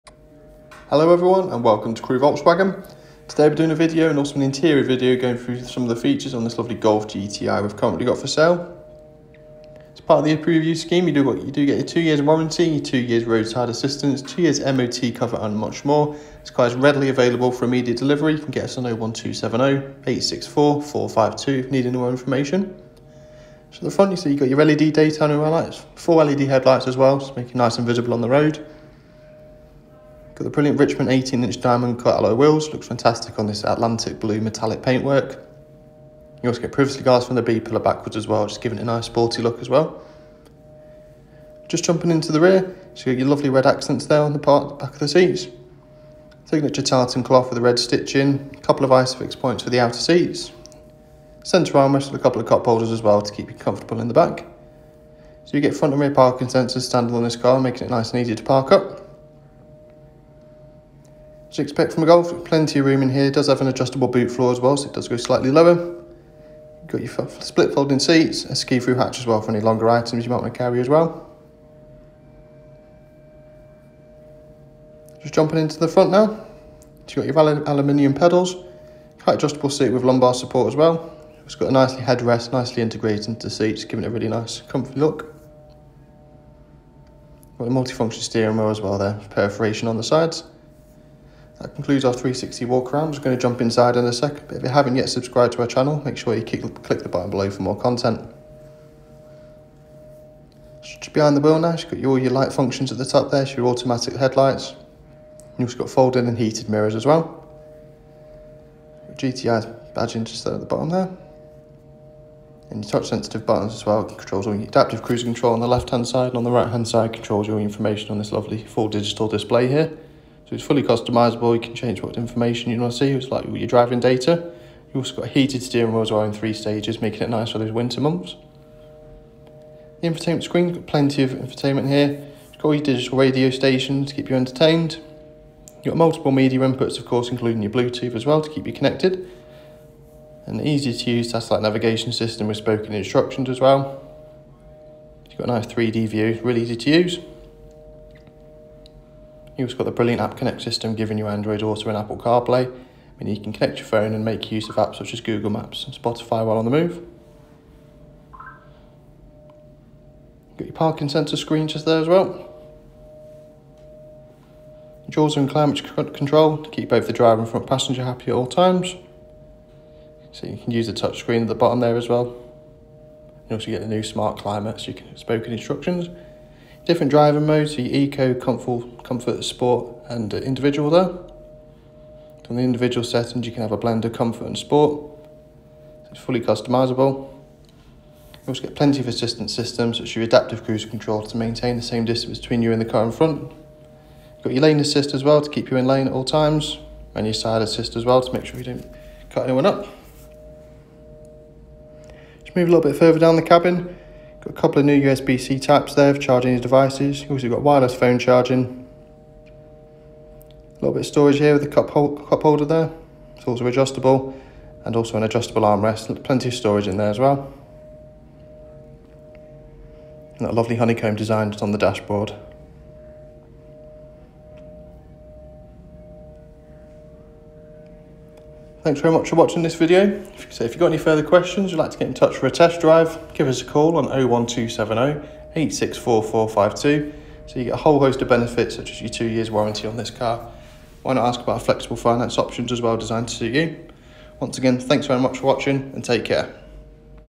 Approved Used Volkswagen Golf 2.0 TSI GTI DSG (s/s) 5dr - DF21VXV
Check out this Approved Used Volkswagen Golf GTI Mk8 5DR Hatchback 2.0 TSI GTI 245PS DSG  currently for sale at Crewe Volkswagen.

Petrol
Automatic
Atlantic Blue Metallic
Auto Lights/Wipers with LED Daytime Running Lights
Front & Rear Parking Sensors
2-Zone Climate Control
Front Assist - City Emergency Braking System
Lane Assist - Lane Keeping System
DAB Radio
Bluetooth

0:00 - Intro
0:20 - Exterior
0:41 - Rear Cabin
1:00 - Boot Space
1:16 - Front Cabin
1:30 - Intro to Interior
1:40 - Behind the Wheel
3:48 - Get in Touch

Available now is this stunning Golf Gti finished in the glorious Atlantic blue metallic paint. This excellent example comes with a host of standard equipment to make every journey a pleasure. to the front you get IQ light matrix LED headlights making quick work of night time driving, you also get front fog lights with signature GTI honeycomb styling. To the side you have 18" diamond cut alloy wheels complementing the paintwork perfectly and also privacy glass aiding in sun protection and enhancing the sporty look. To the rear you have split chrome exhaust tailpipes, further LED technology in the rear lights and an adjustable boot floor making the already practicable boot even more spacious. Inside this GTI you have the stunning red tartan seats, an adjustable armrest providing extra comfort, leather wrapped multifunction steering wheel which is also heated - perfect for those winter months. A sleek touchscreen infotainment system is on hand providing all your entertainment needs including digital radio, bluetooth connectivity for music streaming and telephone calls. You also get Navigation and smartphone connectivity so you can use of apps such as google maps. For more information on this Golf give us a call on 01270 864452 and a member of our friendly sales team will be more than happy to help you further such as book a test drive and introduce you to a range of flexible finance packages to suit you.